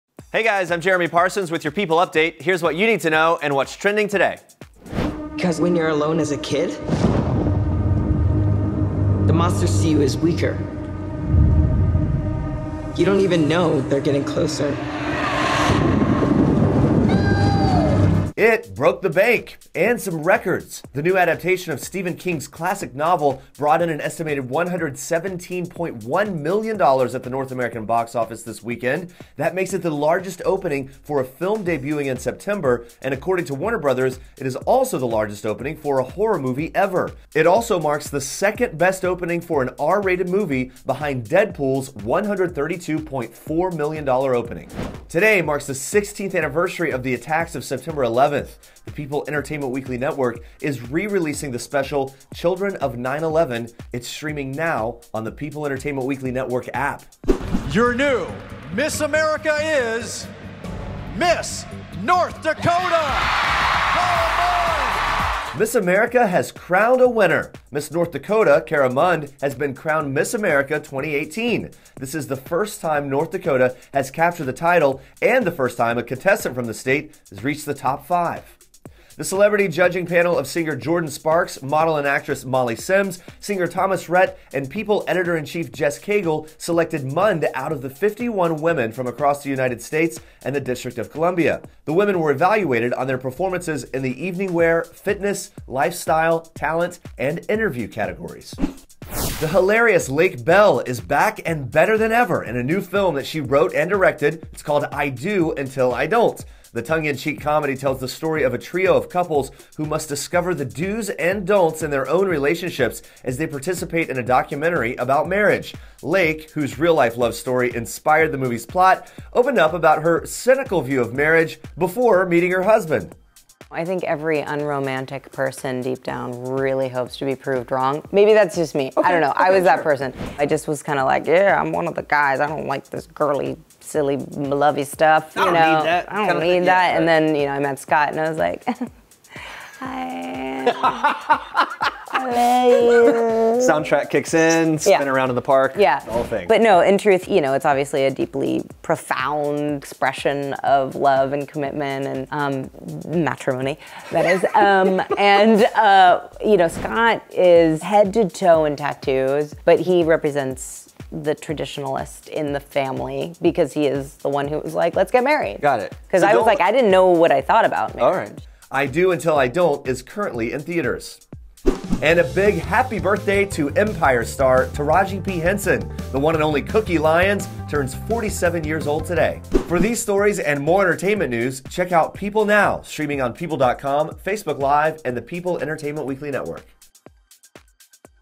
'It' Breaks Box Office Record, Miss North Dakota Is Crowned Miss America! | People Update
The new adaption of Stephen King's classic novel, It, brought in an estimated 117.1 million dollars at the North American box office, making it the largest opening for a film debuting in September. Today marks the 16th anniversary of the attacks of September 11th. The People Entertainment Weekly network is re-releasing their special of Children of 9/11, streaming now on the PEN app. Miss America has crowned a winner! Miss North Dakota Cara Mund has been crowned Miss America 2018. The hilarious Lake Bell is back with her new film "I Do...Until I Don't!" which is currently in theaters. Happy Birthday to Empire star Tarji P. Henson who turns 47 today!